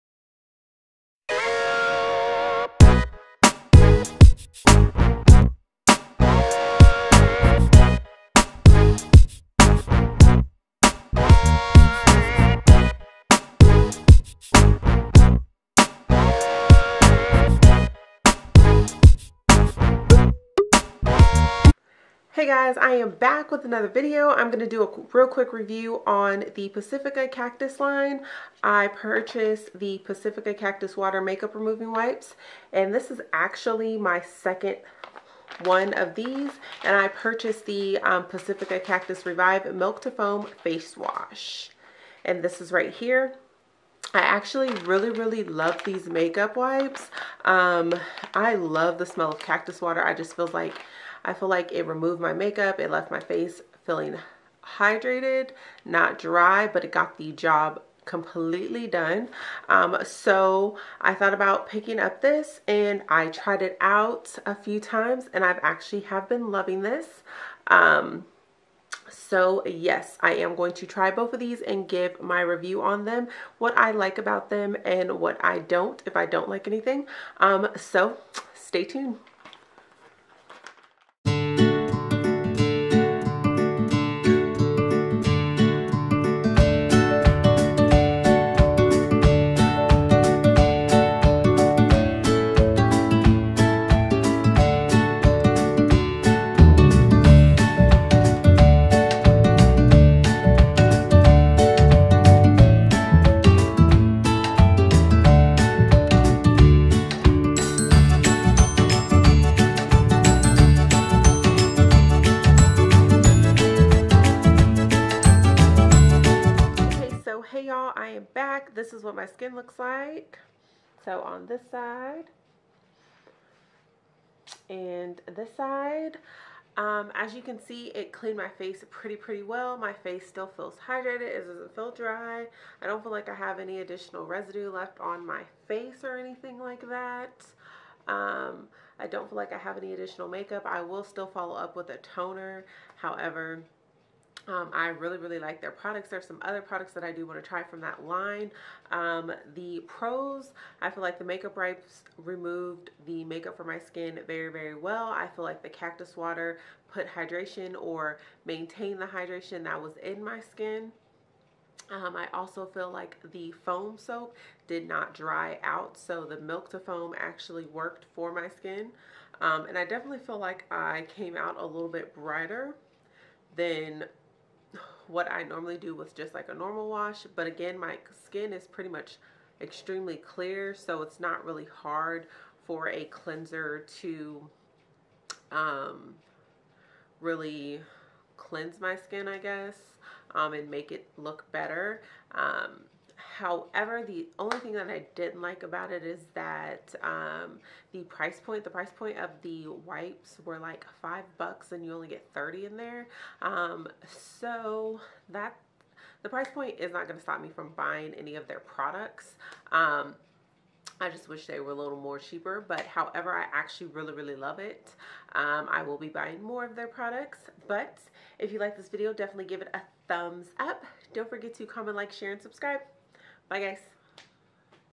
Pacifica Cactus Revive| Demo/ Review
I would recommend this Cleanser to anyone who has OILY or NORMAL skin type

Products in the Video:

Pacifica Cactus Revive Milk to Foam Face Wash

DONT forget to GIVE this video a T H U M B S U P and S U B S C R I B E